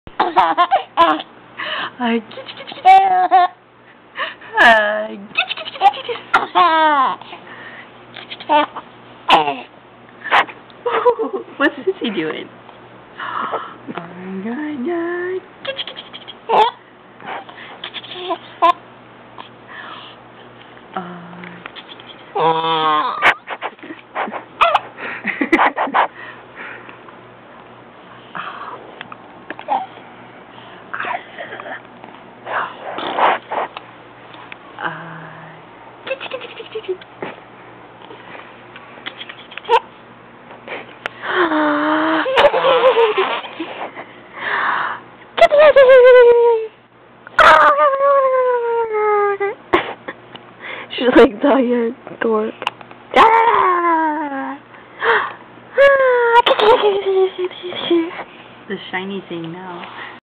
Phoebe Fae Rose
A few months old.
She's giggling.